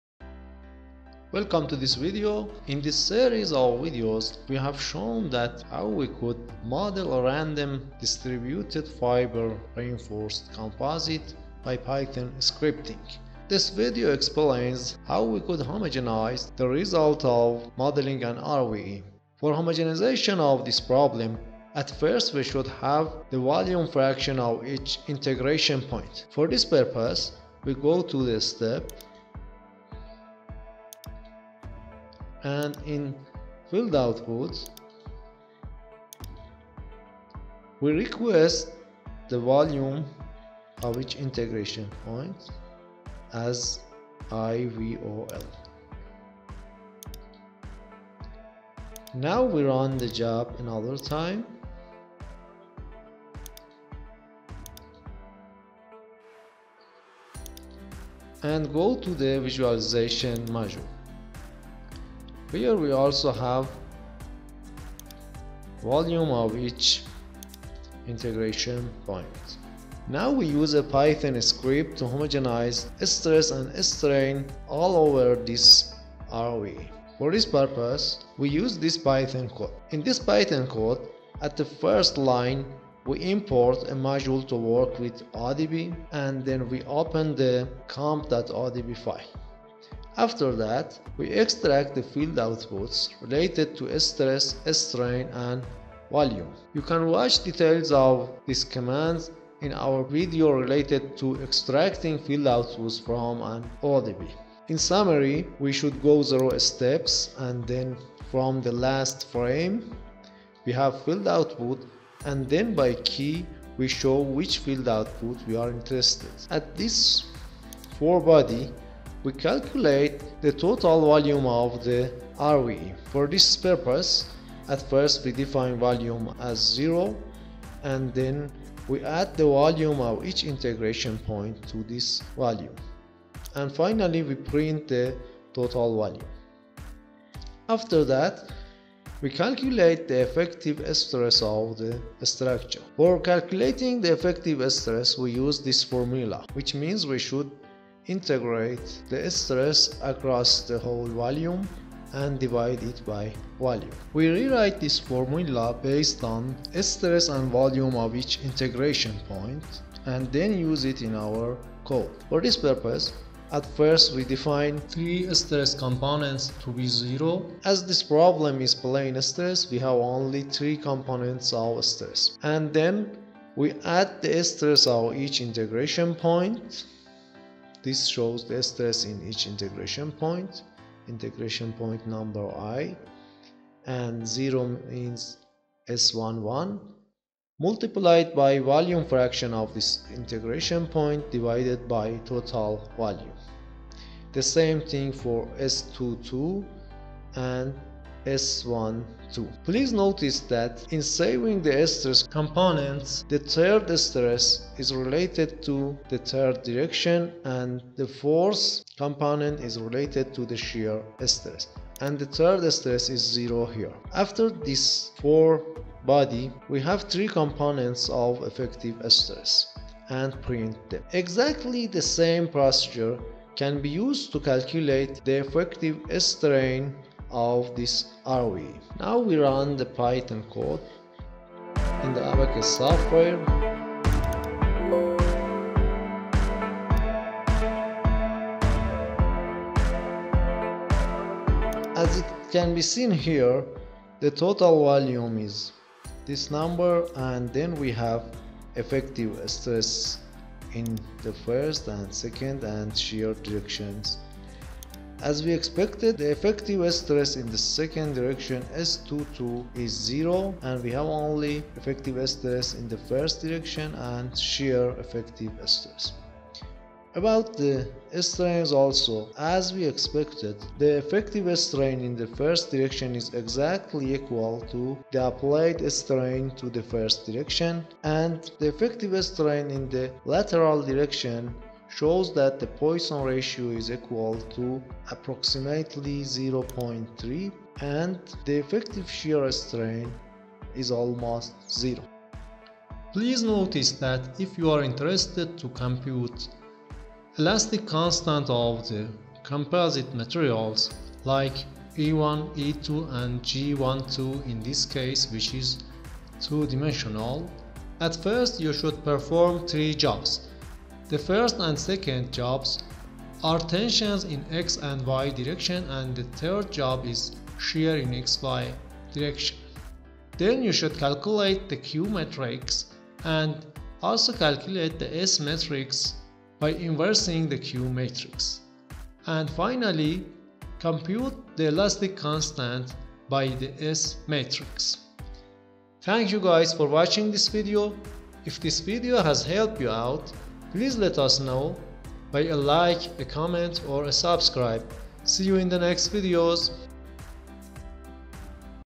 Effective moduli by homogenization of stress and strain of RVE/UC by Python/Abaqus
In this video homogenization of stress and strain of RVE/UC of a random distributed composite RVE is explained. The modeling is done in Abaqus, and using a Python code effective stress and strain are calculated. Then calculating effective Moduli is explained. In this video you can find out:
1- How to request volume fraction of integration points?
2- How to calculate effective strain?
3- How to calculate effective stress?
4- How to calculate effective moduli?